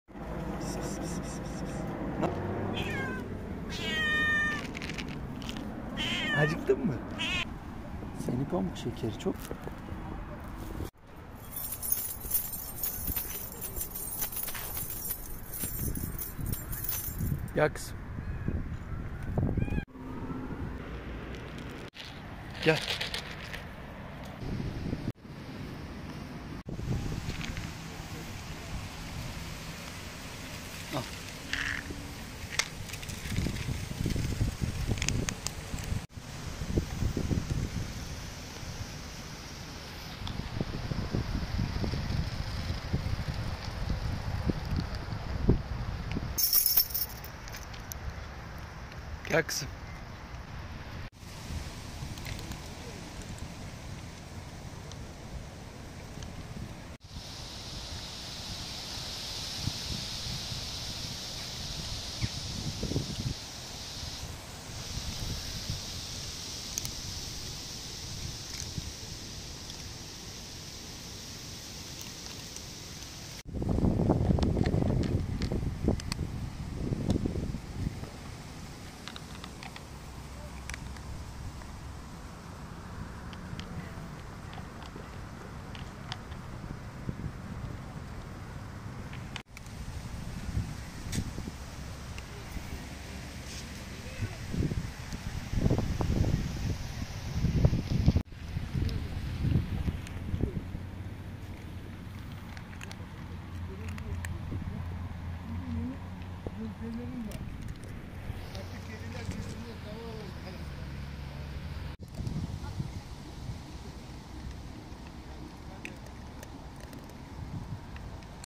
Feeding hungry, homeless cats, All so cute 🥰 cat vlog 27
My daily antidepressant is spending time with those angels.